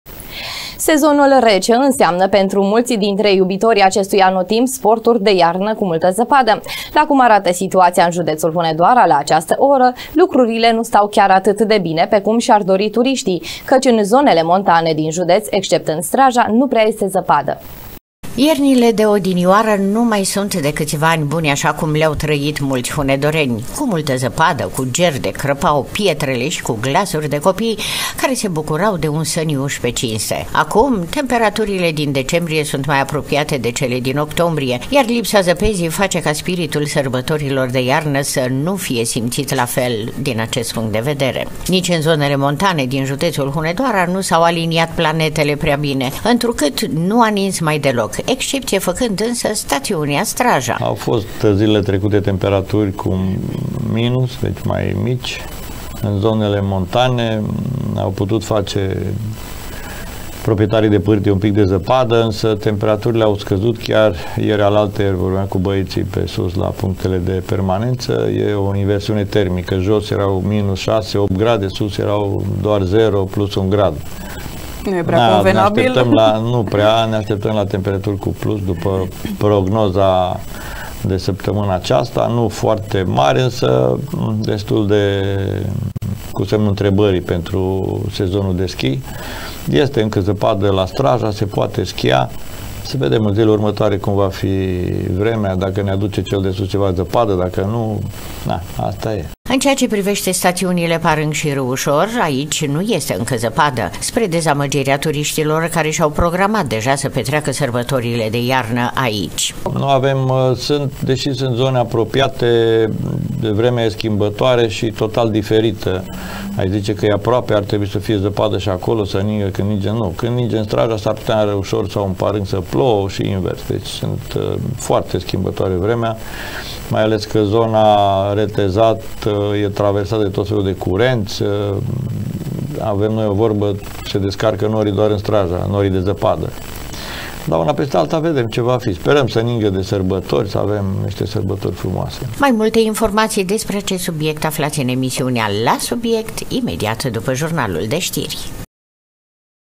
ZĂPĂDĂ MAI PUȚINĂ, ÎN ACEST FINAL DE AN, ÎN ZONELE MONTANE A JUDEȚULUI
SEZONUL RECE ÎNSEAMNĂ PENTRU MULȚI DINTRE IUBITORII ACESTUI ANOTIMP SPORTURI DE IARNĂ CU MULTĂ ZĂPADĂ. LA CUM ARATĂ SITUAȚIA ÎN JUDEȚUL HUNEDOARA, LA ACEASTĂ ORĂ, LUCRURILE NU STAU CHIAR ATÂT DE BINE PE CUM ȘI-AR DORI TURIȘTII, CĂCI ÎN ZONELE MONTANE DIN JUDEȚ, EXCEPTÂND STRAJA, NU PREA ESTE ZĂPADĂ.